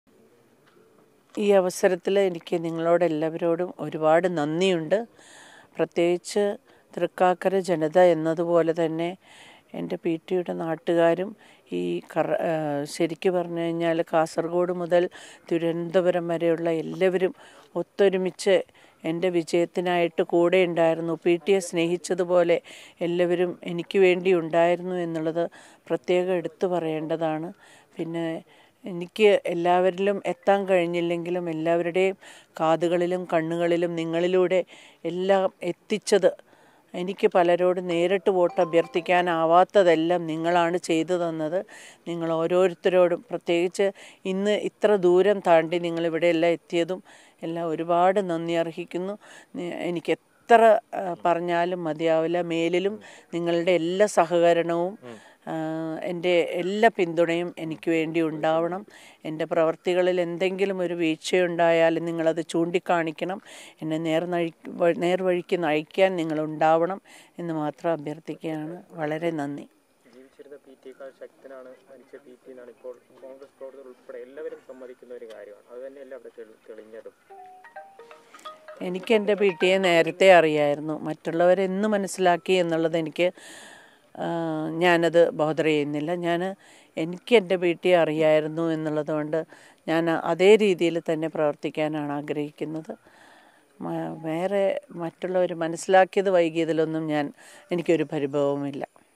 'പി ടിയെ സ്നേഹിച്ച എല്ലാവരും എനിക്കൊപ്പമുണ്ടായിരുന്നു'; നന്ദി പറഞ്ഞ് ഉമാ തോമസ് | Mathrubhumi News
തിരഞ്ഞെടുപ്പ് ജയത്തിൽ എല്ലാവർക്കും നന്ദി അറിയിച്ച് ഉമാ തോമസ്.

Happy viewing!